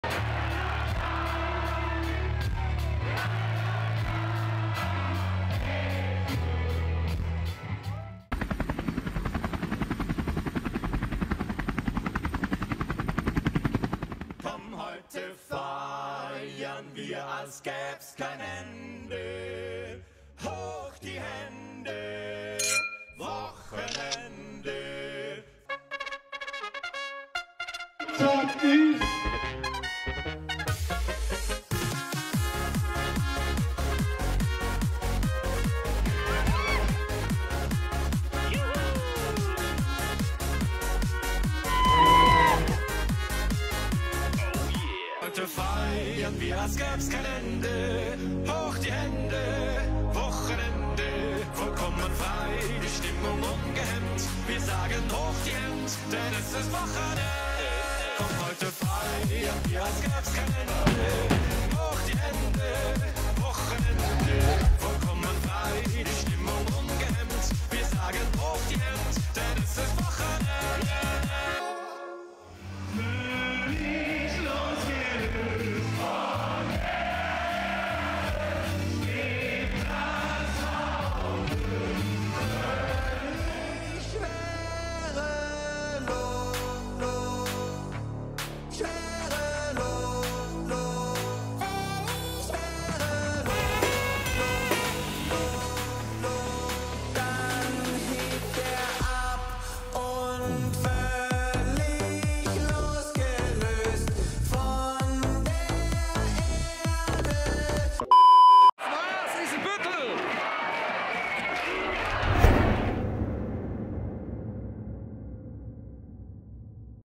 Frühlingswiesn Isenbüttel bei ROTH Catering 2023
FRÜH­LINGS­WIESN IN ISEN­BÜTTEL
Die Frühlingswiesn garantiert eine Mordsgaudi und ist das größte „Oktoberfest“ der Region Gifhorn.

Mitten im Frühling feiern pro Abend ca. 1500 Besucher in Dirndl und Lederhosen und tanzen zur Live-Musik der populärsten Oktoberfest-Band der Welt, der Münchner Zwietracht, auf den Bänken. Abgerundet mit deftigen Schmankerln, Dekoration im bayerischen Stil und schmackhaften Wiesnbier, das mit einem offiziellen Fassanstich zum Fließen gebracht wird, ist das Fest ein authentisches Wiesnerlebnis.